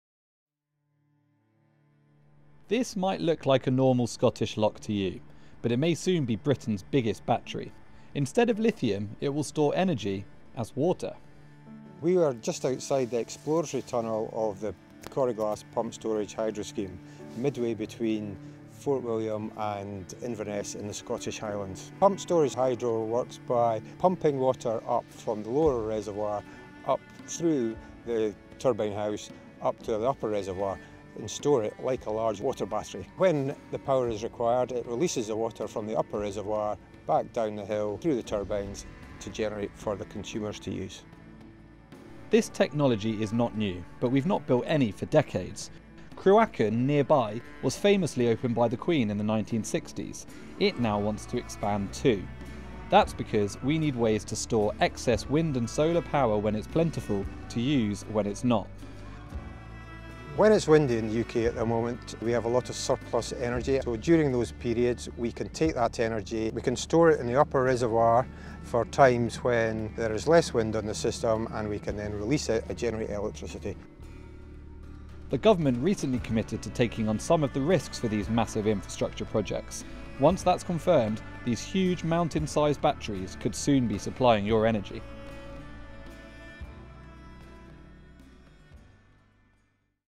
Why this mountain could soon be Britain's biggest battery
This Scottish mountain could soon be Britain's biggest battery.
It will store renewable energy when its plentiful, to use when it's not.
A critical solution for our future clean grid.